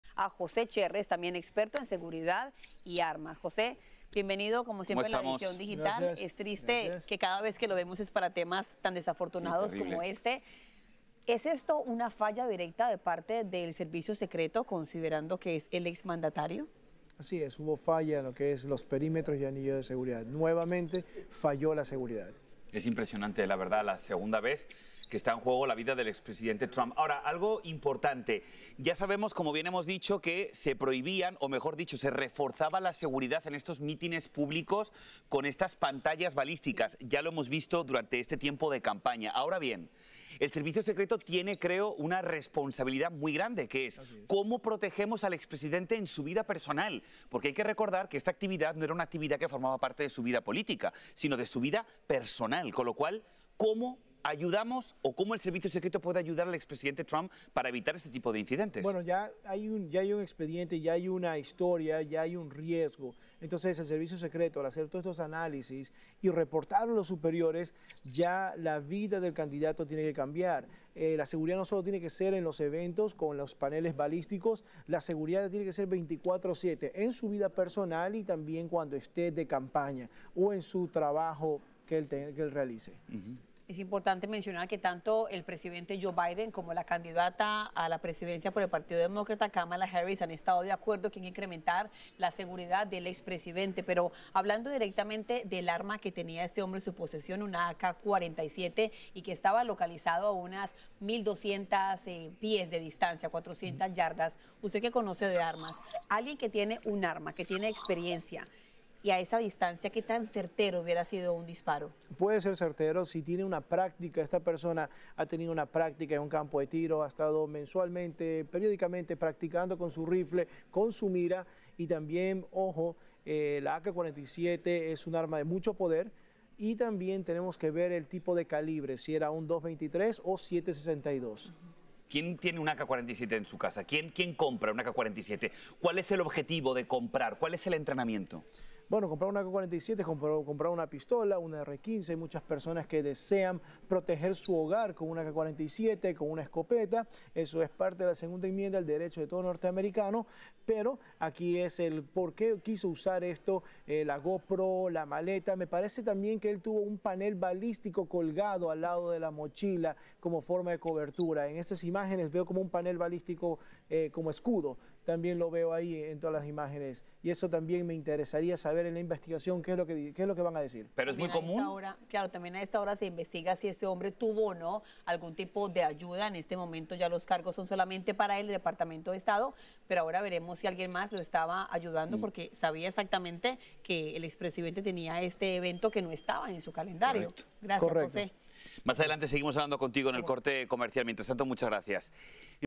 ⚠️⚠️ Fallos del Servicio Secreto: Caso del Hombre Armado en la Propiedad de Trump
En esta entrevista en Univision, el experto en seguridad y armas, Jose L. Cherrez, analiza el incidente donde Ryan Wesley Routh, un hombre armado con un rifle, permaneció oculto durante 12 horas cerca del campo de golf de Donald Trump en Florida. Cherrez ofrece su análisis sobre las serias fallas del Servicio Secreto en este incidente y los riesgos que enfrentan las propiedades de alto perfil. Routh, con antecedentes criminales, fue arrestado tras apuntar a un agente con un arma de alto poder. Cherrez explica qué pudo hacerse mejor y cómo prevenir futuros errores en la seguridad presidencial.

Este video ofrece una perspectiva profesional sobre cómo los protocolos de seguridad fallaron en este caso crítico y qué lecciones pueden aprenderse.

Mis Libros

Ropa y accesorios tácticos los pueden buscar en el siguiente enlace de Amazon: